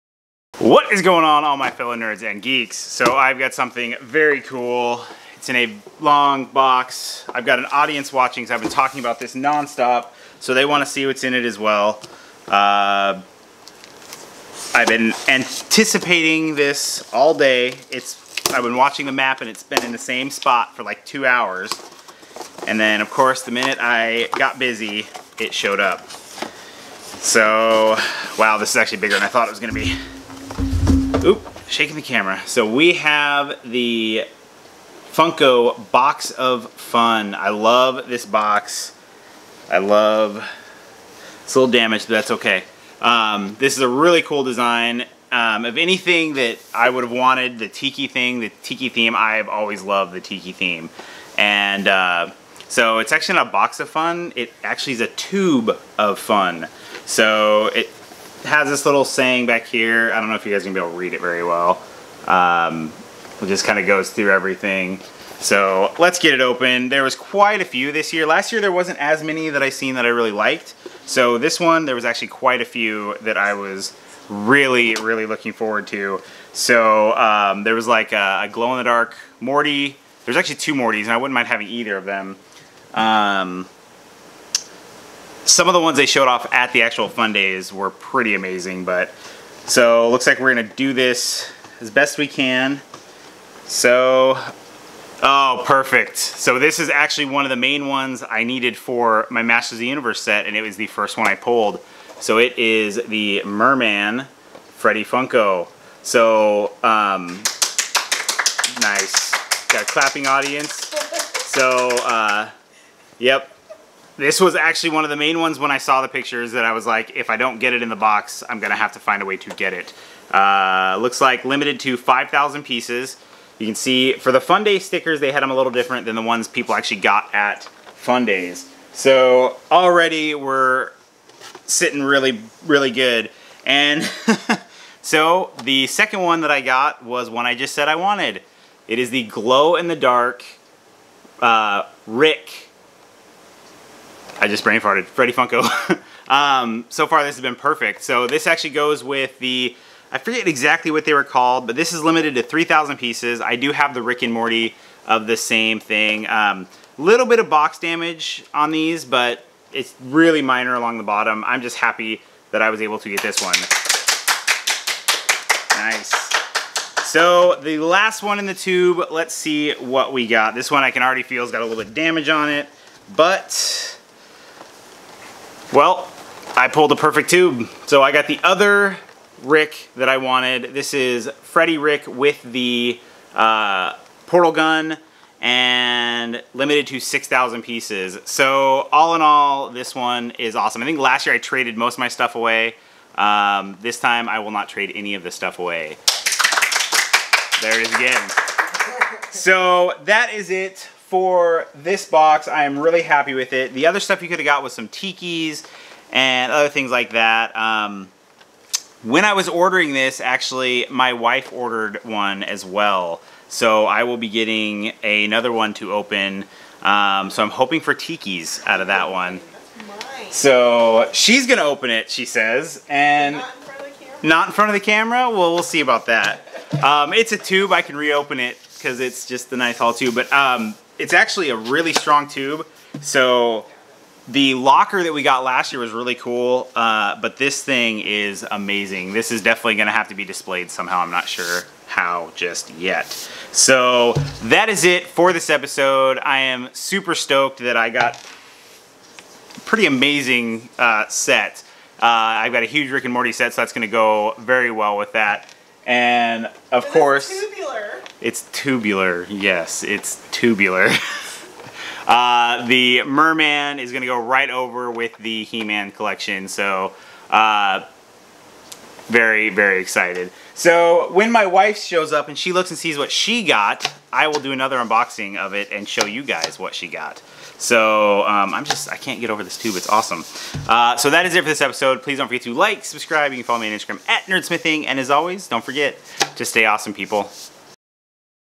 Funko Tiki Box of Fun 2019
This is my Funko Fundays Tiki box of fun unboxing I got a great box! It couldn't fit my collection any better.

As always stay AWESOME people!